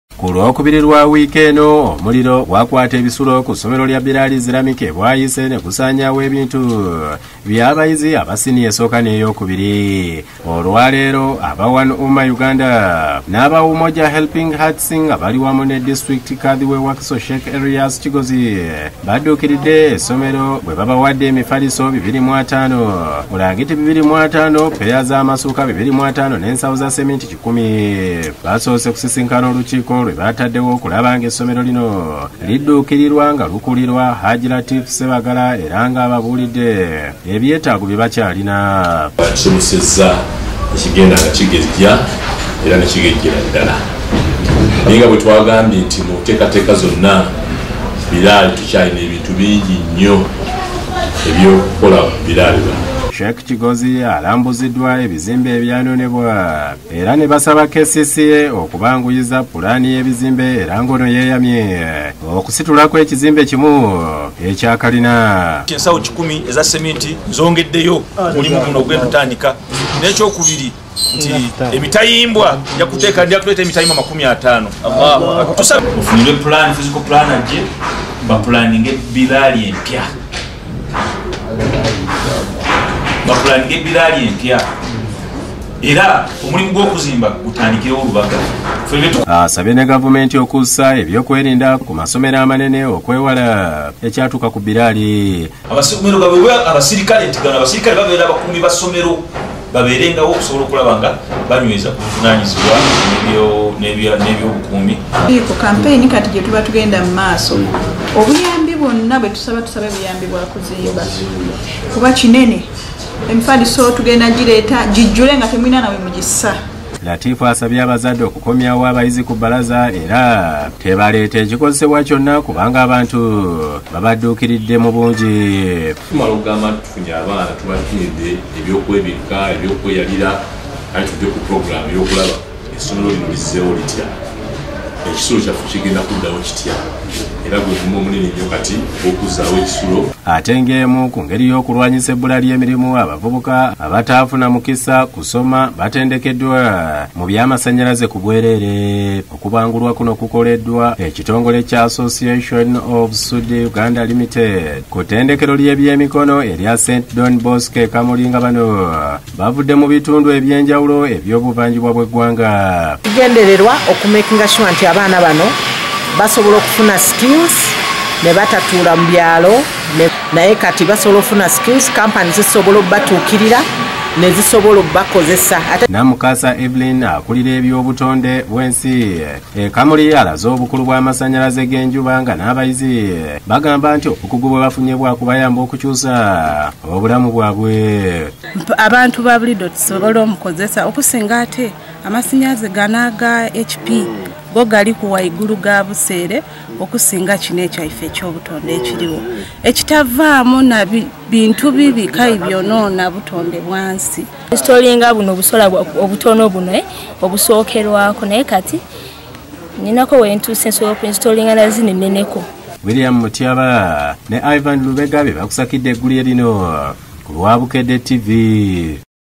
Agataliikonfuufu: Essomero lya Biral lidduukiriddwa
Liweereddwa ebikozesebwa  Abayizi baddamu ku bb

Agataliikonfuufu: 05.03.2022
Kuteesa G.W